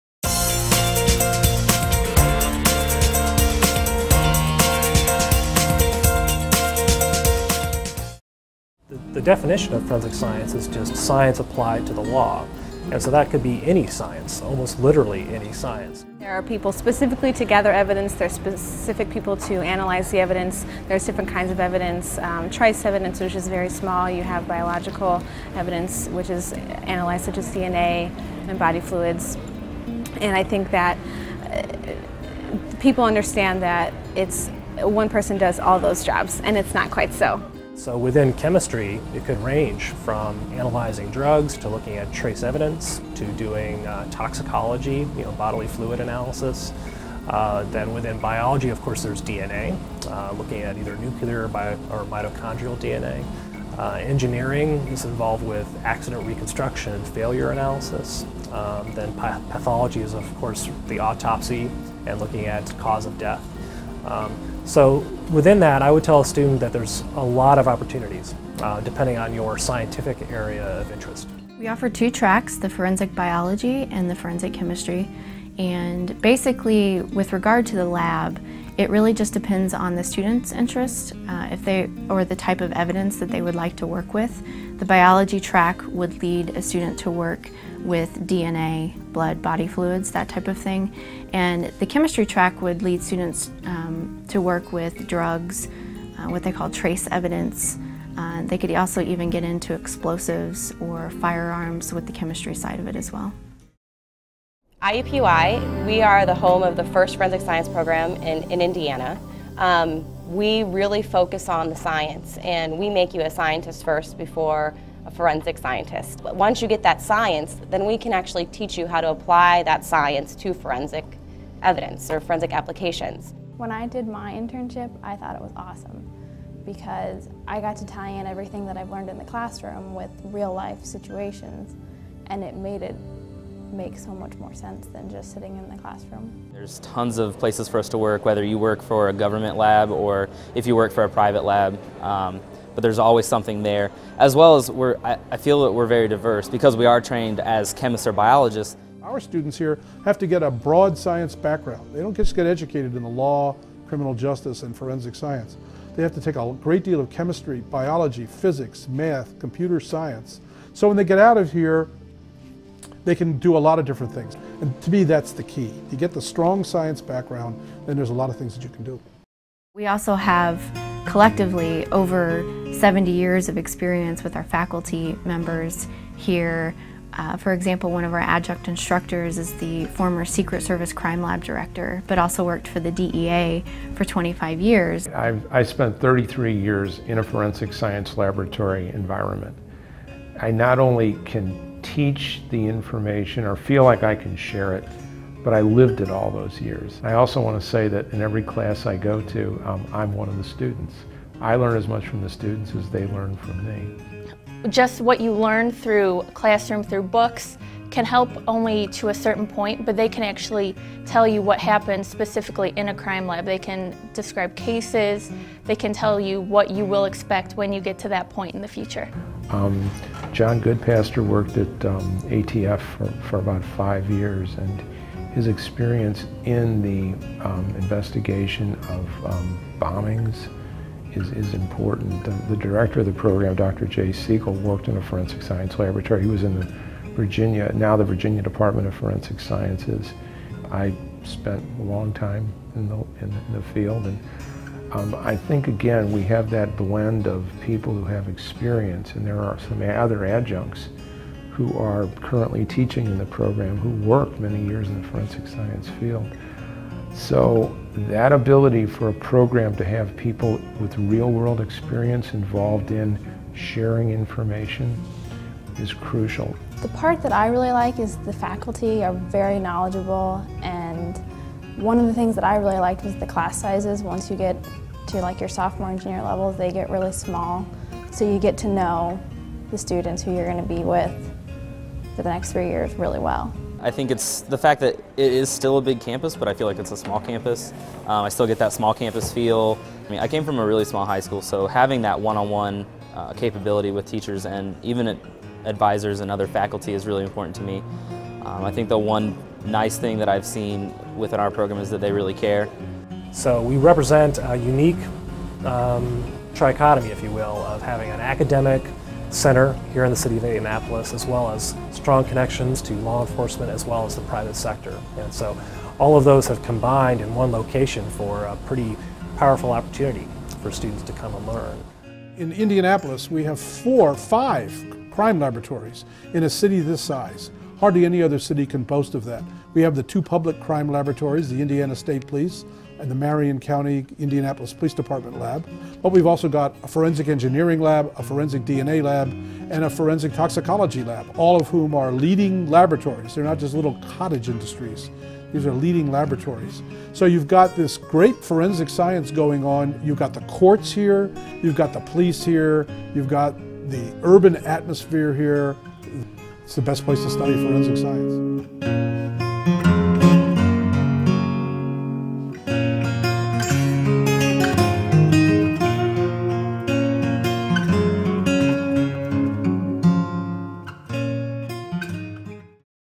Discover Forensic Science @ IUPUI School of Science
Explore how science is applied to the law through integrated classroom and real-world experience at the only nationally accredited forensic science program in the state of Indiana.